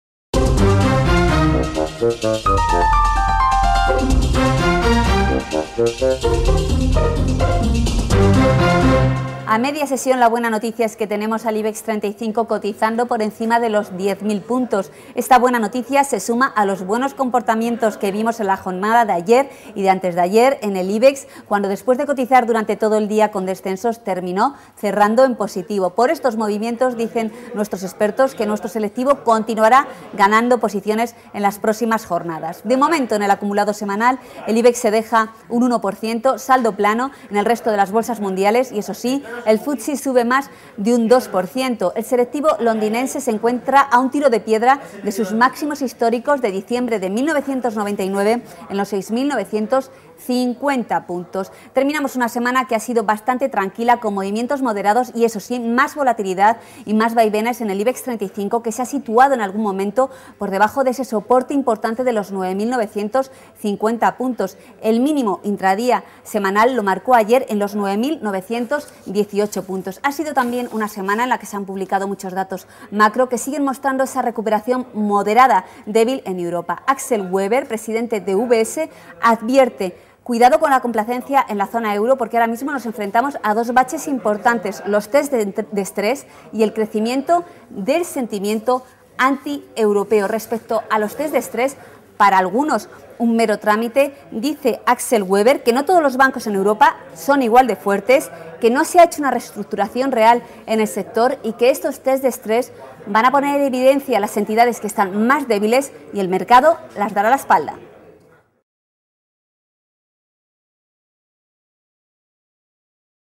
Con lupa
La recuperación en la Zona Euro no será tan lineal ni tan continuada como algunos esperan. Así lo advierte hoy Axel Weber, presidente de UBS, quien deja claro que hay que evitar la complacencia. Europa, dice, se enfrenta a dos baches a día de hoy: los tests de estrés (que pondrán en evidencia a aquellos bancos que están mal) y el crecimiento del sentimiento anti-europeo, cada vez mayor.